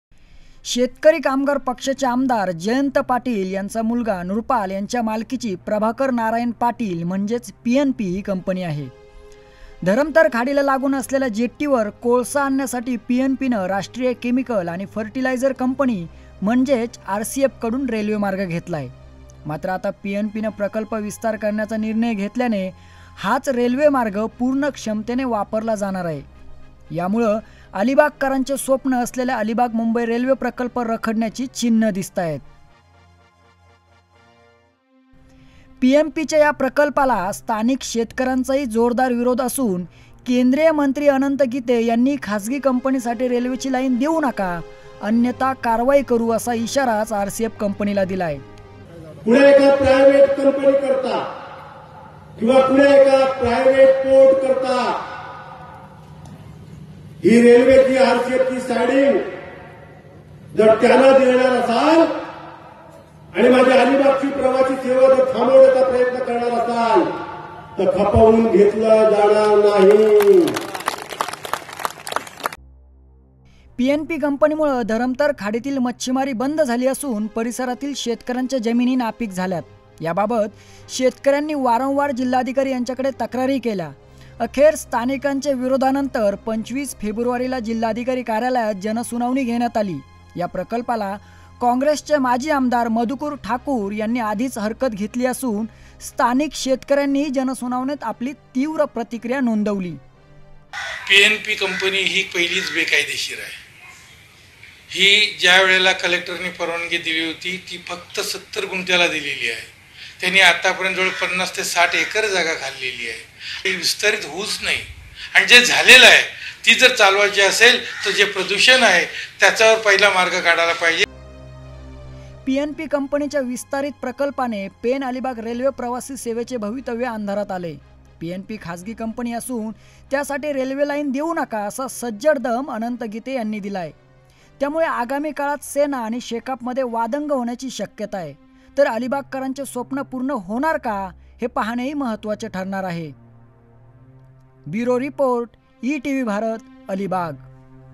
खासगी कंपनीसाठी रेल्वे मार्गाची स्लायडिंग दिली जाणार नाही - अनंत गीते
रायगड - पीएनपी या खाजगी कंपनीला किंवा पोर्टला कोणत्याही परिस्थितीत रेल्वे मार्गाची स्लायडिंग दिली जाणार नाही. जर कोणी अलिबाग प्रवासी रेल्वे सेवा थांबविण्याचा प्रयत्न करीत असेल, तर खपवून घेतले जाणार नसल्याचा इशारा केंद्रीय मंत्री अनंत गीते यांनी पीआनपीसह आरसीएफला दिला आहे.